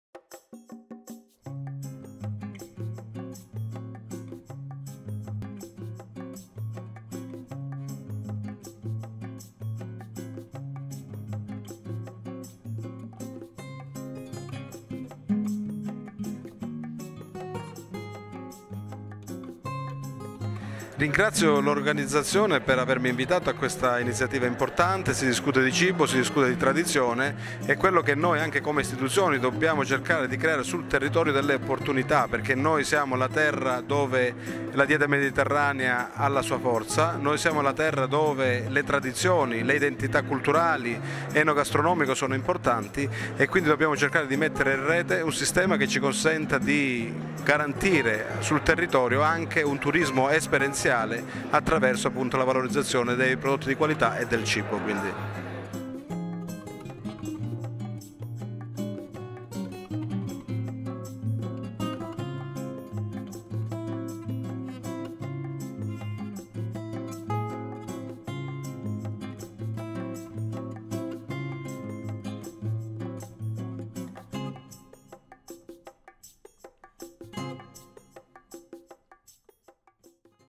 Intervista a Salvatore Solano - Vibo Valentia in Festa 2019
Intervista a Salvatore Solano, Presidente Provincia di Vibo Valentia - Vibo Valentia in Festa 2019

Riprese a cura di Antonio Abbruzzese (Associazione Fiore di Lino)